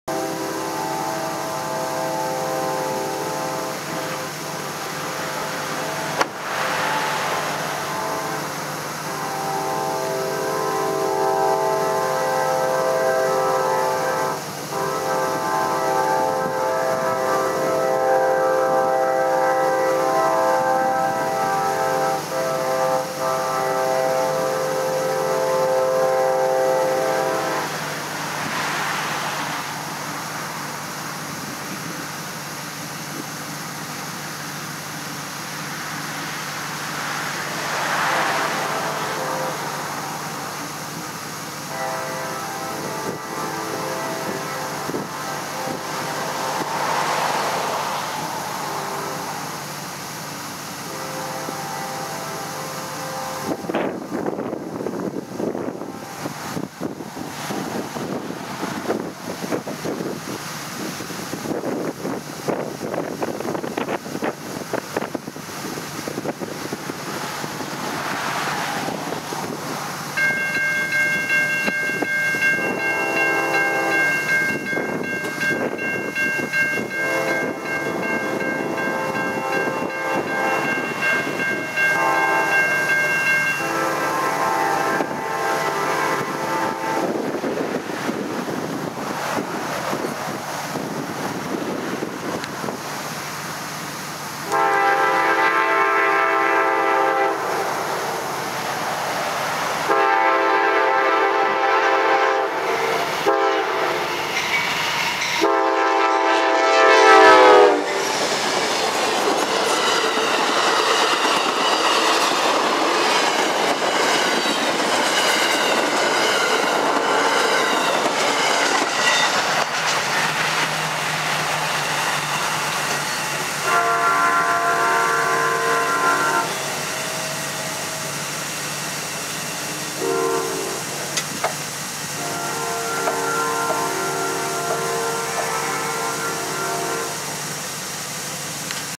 The Amtrak Crescent #19 With Awesome Horn! Douglasville Ga 10-20-2013© HD
9:11AM in Douglasville,Ga is the Amtrak Crescent #19 with an awesome sounding horn!

Recorded by Gene and Tommy Hale